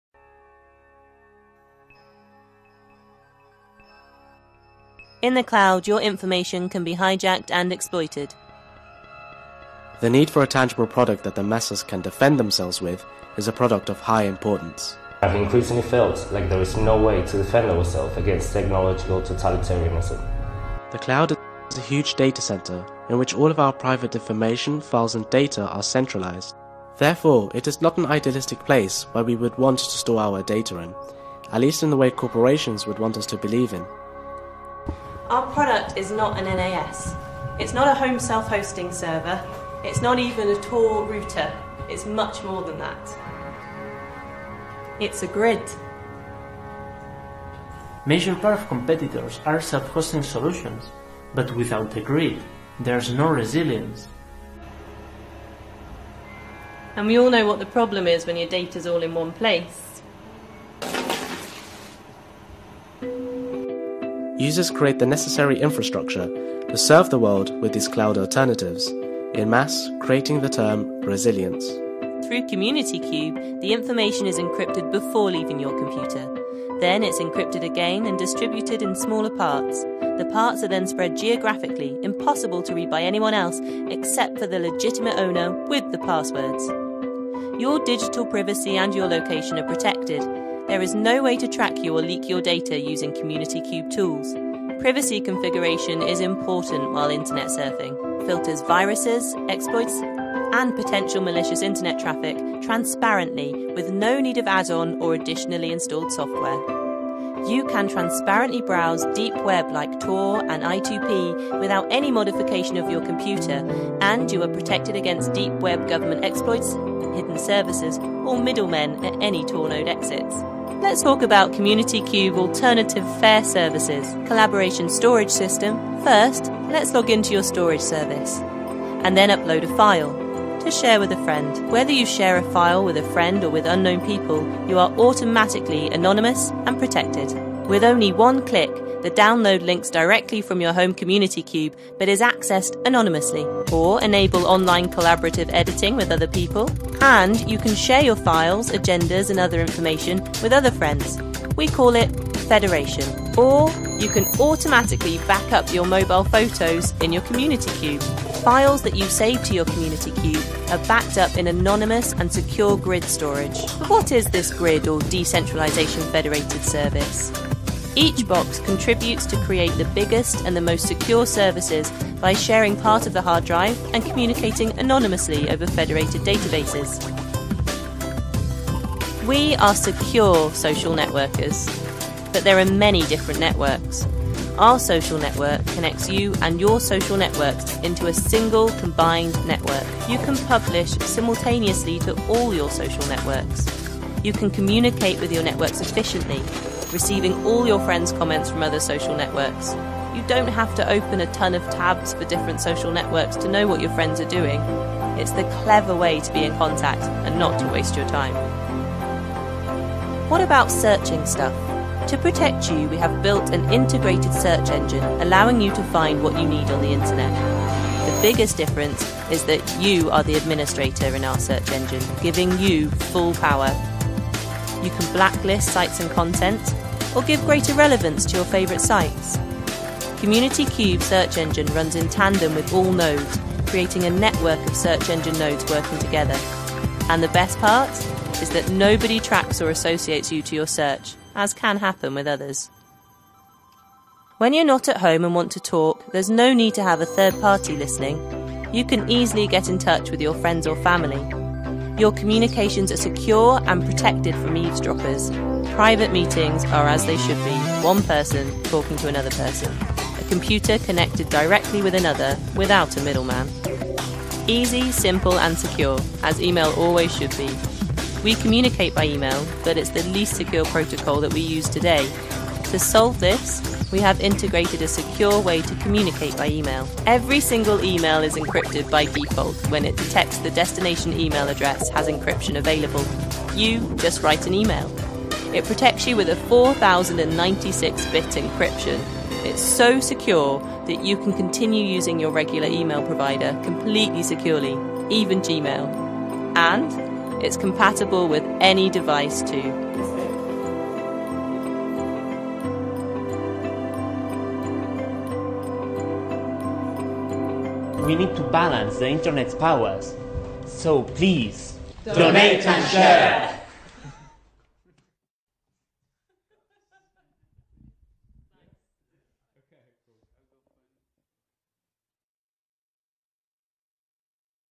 CommunityCube Kickstarter
Community Cube is a plug and play, open source, small server designed to build a cooperative and fair Internet where users’ privacy is protected. Community Cube is a one-time-payment product with no hidden fees. It's both a server and free secure services. This one-to-all, all-to-one model provides an ideal platform that renders every Internet service you might want, but without the risk of being spied on by the government, corporations, hackers and malware.